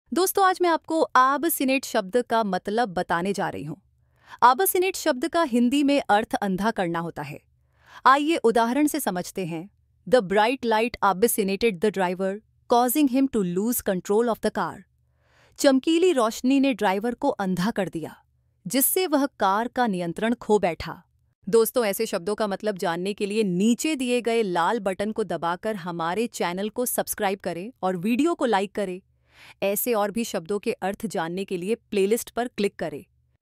Abacinate meaning in Hindi | Abacinate ka matlab kya hota hai | English to hindi
क्या आप "Abacinate" शब्द का मतलब हिंदी में जानना चाहते हैं? इस वीडियो में हम "Abacinate" का हिंदी में अर्थ, उच्चारण और उसके उपयोग को समझेंगे। हम इसे "अभसिनेट" के रूप में भी उच्चारित कर सकते हैं। इस वीडियो में हम विस्तार से चर्चा करेंगे कि "Abacinate" का मतलब क्या होता है और इसे वाक्यों में कैसे उपयोग किया जा सकता है। इसलिए, अगर आप इंग्लिश से हिंदी में शब्दों का अनुवाद करना सीखना चाहते हैं, तो इस वीडियो को अंत तक देखें।

इसमें हम निम्नलिखित बिंदुओं पर ध्यान देंगे:
- Abacinate का हिंदी में अर्थ
- Abacinate का सही उच्चारण
- Abacinate का उपयोग वाक्यों में